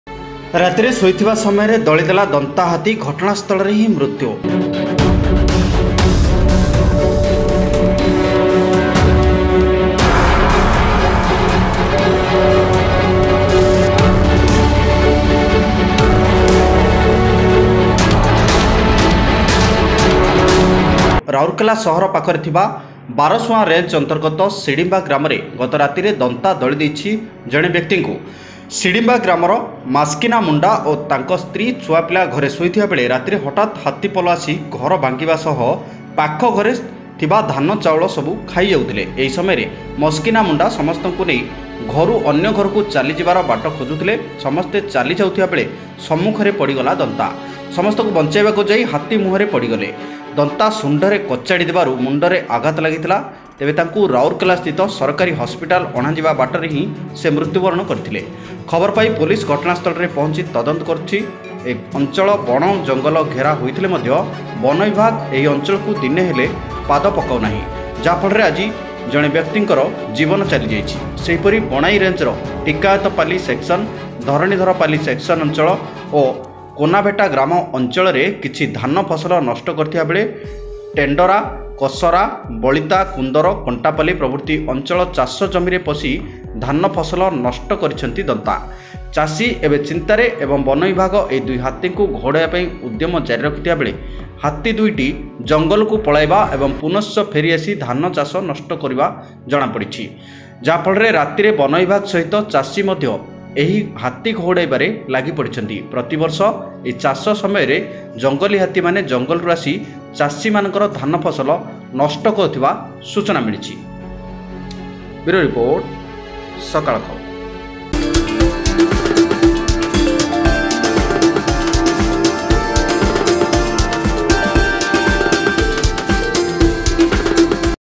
ରାତିରେ ଶୋଇଥିବା ସମୟରେ ଦଳି ଦେଲା ଦନ୍ତା। ଜଣେ ମୃତ।
Sakala Khabar